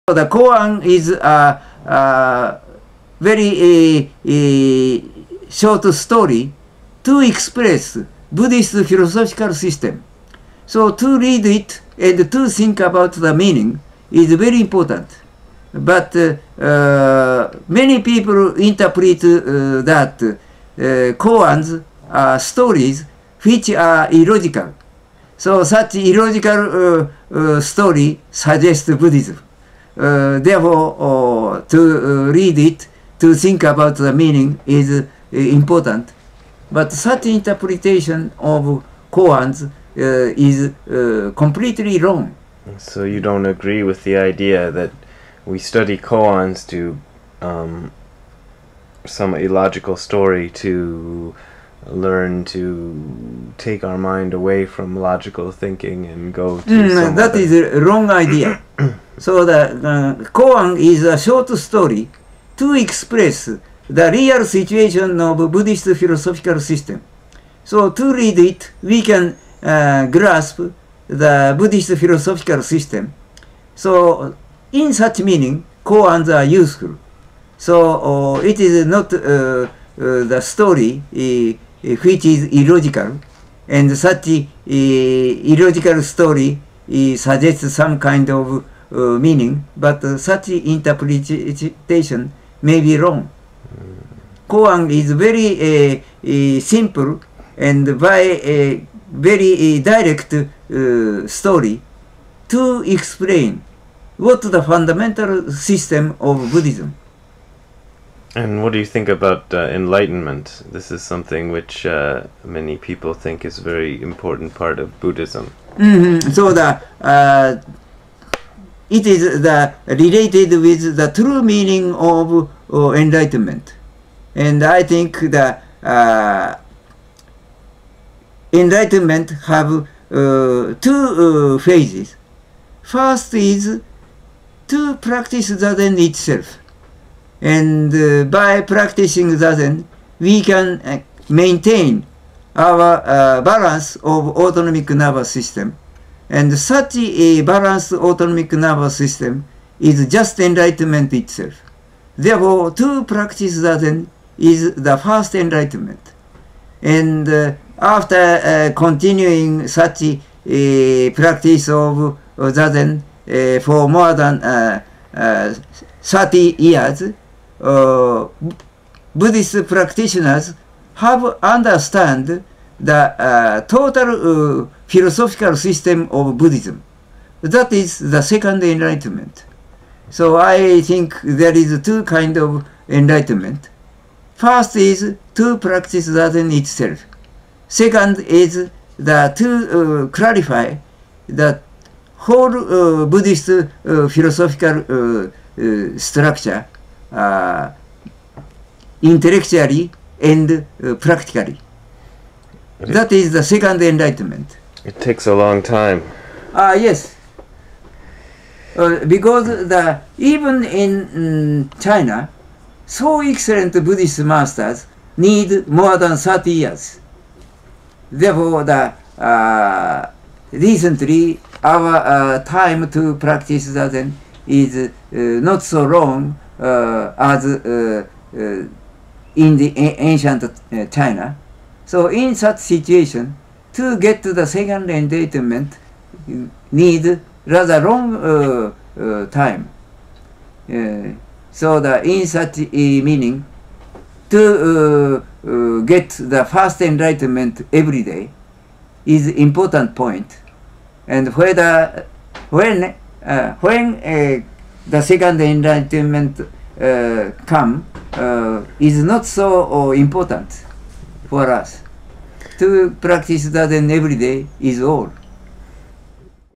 Gudo Nishijima Roshi on Koans and Enlightenment
Gudo Nishijima Roshi translator of Dogen's Shobogenzo talks about Zen koans and their relationship to Enlightenment.